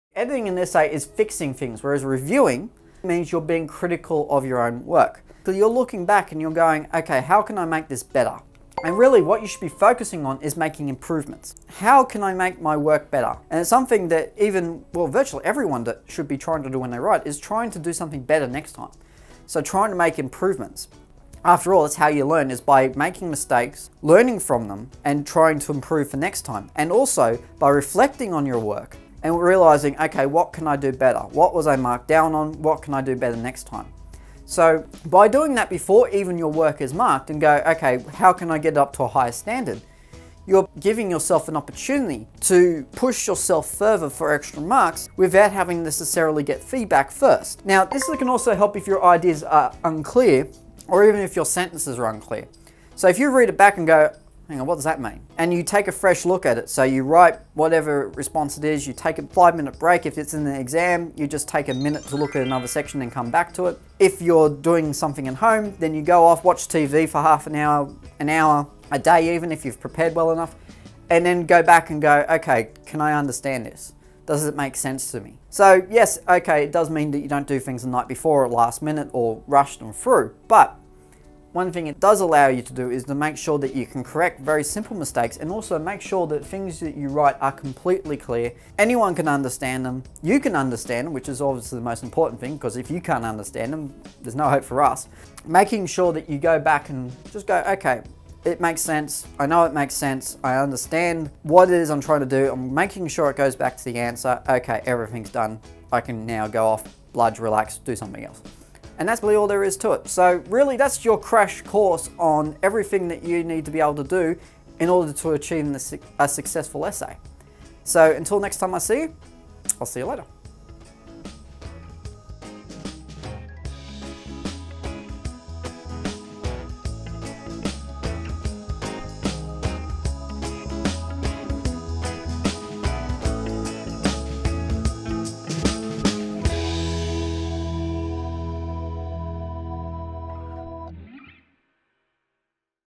💯 Techniques for Essay Success #7/7 How to Review Essays
6. Review, Not Edit
Editing an essay is ‘fixing’ things; whereas ‘reviewing’ indicates you are being critical of your own work. By reviewing your work, you should concentrate on making improvements after your initial draft. This can help if your ideas are unclear or if your sentences are unclear.